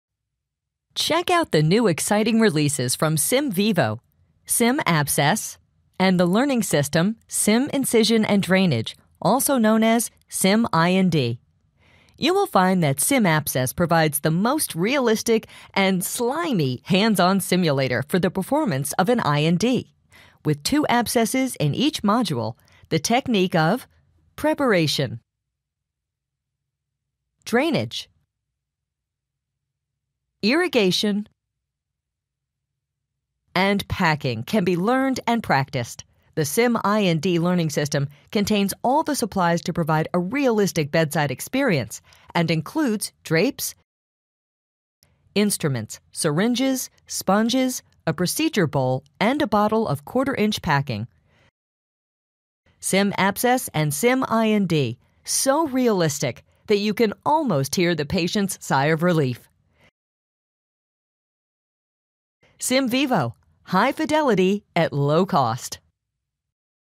Sim*Abscess and Sim I&D - What makes them so unique?
This brand-new personal learning system includes everything to create a high-fidelity learning environment for incision and drainage. From start to finish, there isn’t a more life-like scenario in a non-clinical setting. While an I&D may seem simple, sterility and safety measures dictate practice in a simulated situation.

Beginning with an evaluation of the inflammation, notice the true look and feel of a tender abscess. Once the abscess is located, you’ll drape the area and then carefully inject "anesthetic" using a 3mL syringe. You’ll then proceed to incise, drain, irrigate, and finally finish by packing the open wound. Sim I&D offers a truly realistic experience.

Our abscess is made with a fully proprietary blend of non-toxic material to create the texture and consistency of pus, offering unmatched realism in pop, squirt, and ooze.

High-fidelity learning environment: With Sim I&D, you'll be able to practice your skills in a realistic, non-clinical setting.
Sterility and safety: Our system allows you to practice an I&D without compromising on sterility or safety measures.
True-to-life experience: Our proprietary blend of non-toxic materials creates a realistic simulation of an abscess, allowing you to experience every aspect of the procedure.
Practice and perfect: With Sim I&D, you can practice and perfect your skills as needed to feel confident performing I&D on real patients. (Additional Sim*Abscess boards are available for separate purchase)
Abscess board has a 60-day shelf life and product may begin to degrade beyond 60-days. Each board is made to order, please allow extra processing time.

 Please note - allergen warning: Abscess board contains latex material.